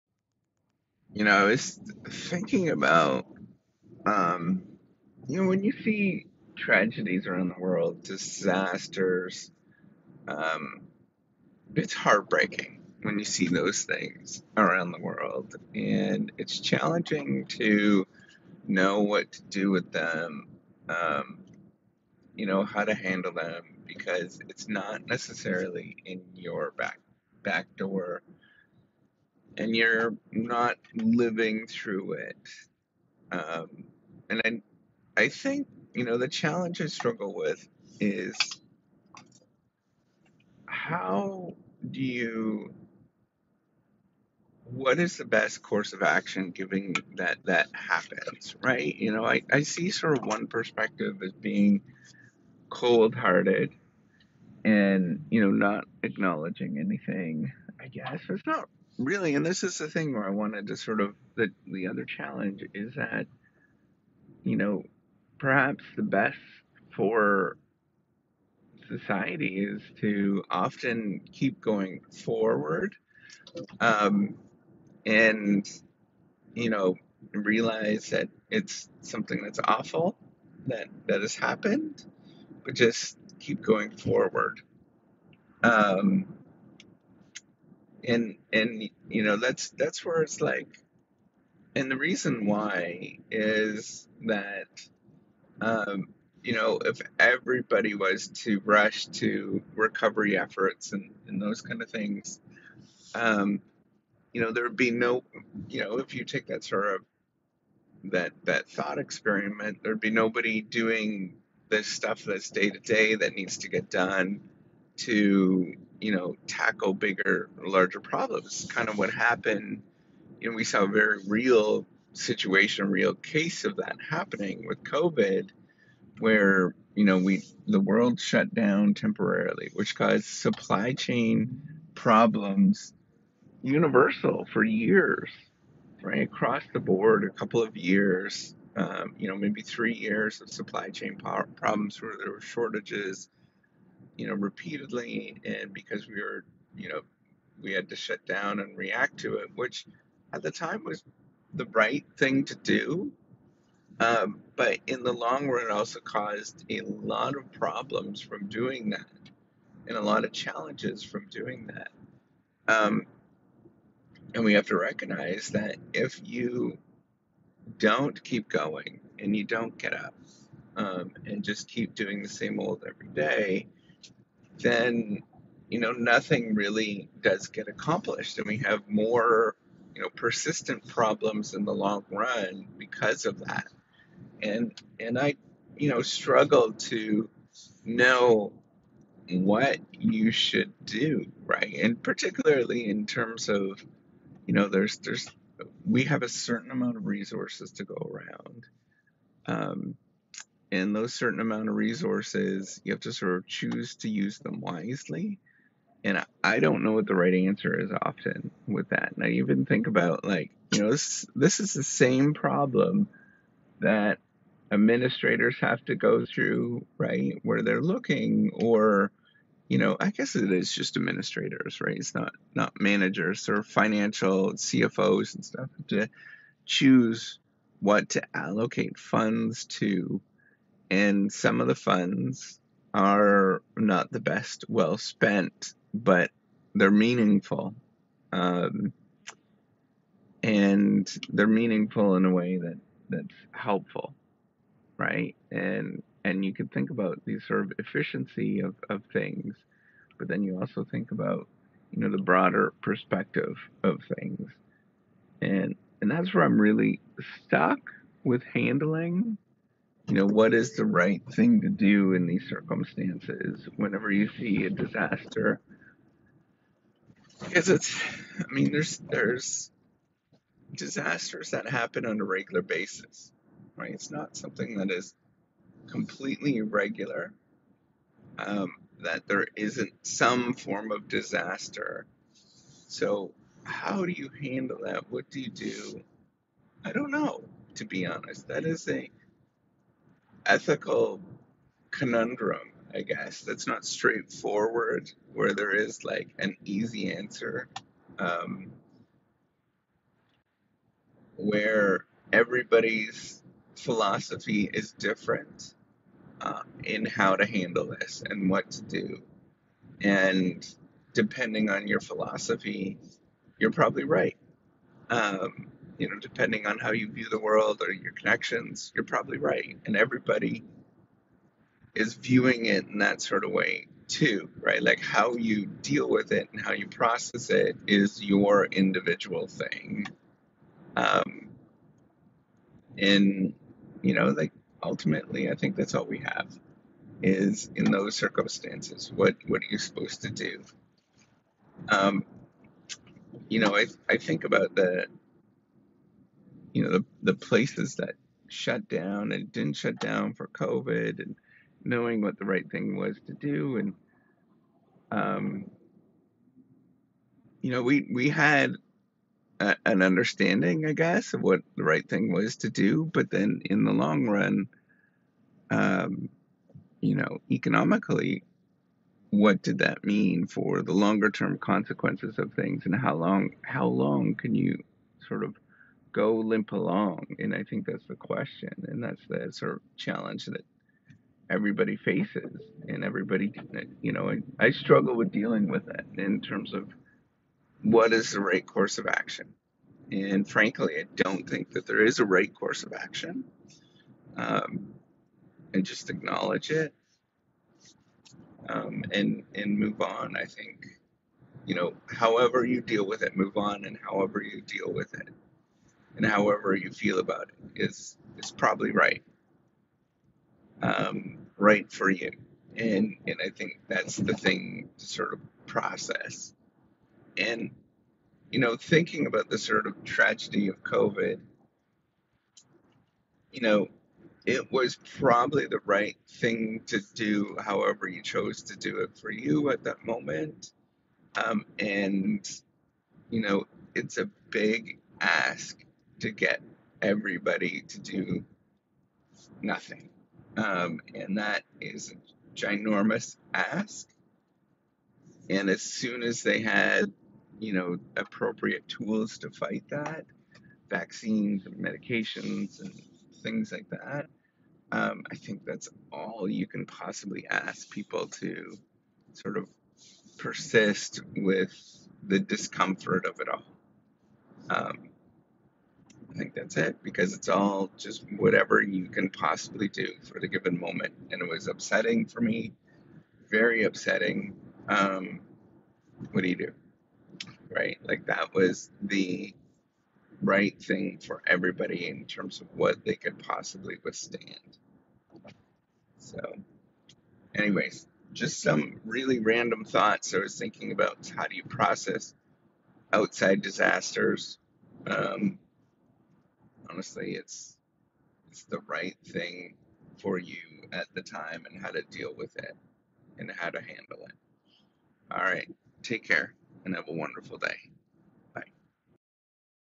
How to process and think about disasters
How to process and think about disasters that you see. The challenge is to know what you should do in response. I think any possible response in terms of the way that you choose is probably the appropriate response at the time. Each person deals with disasters in their own separate way, and processes them in their own way of dealing with it.

While I think, sometimes it appears upsetting from the outside, there’s not really a good course of action ever makes sense. Each individual choice likely makes the most amount of sense for that.